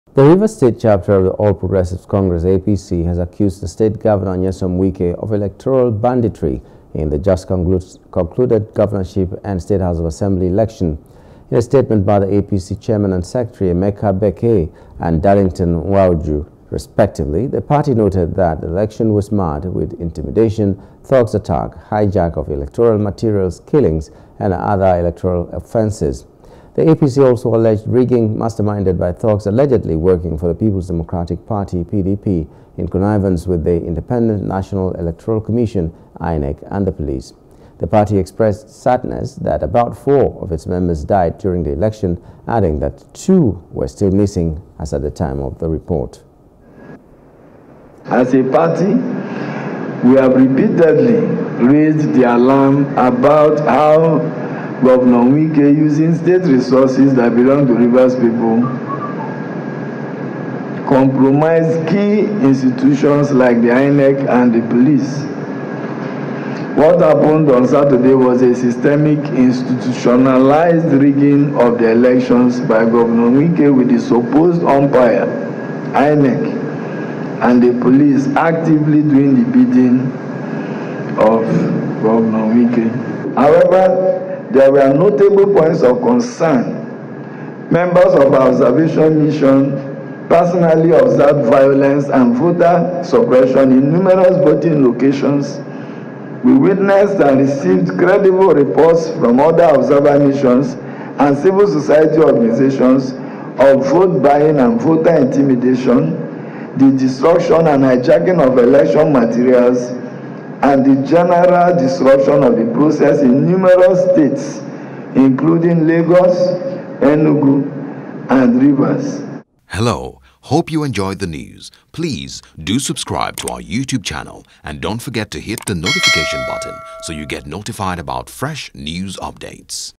Governorship Election: A.P.C Accuses Wike Of Electoral Banditry In Rivers
The Rivers State chapter of the All Progressives Congress (APC) has accused the state governor, Nyesom Wike, of electoral banditry in the just concluded governorship and state house of assembly election.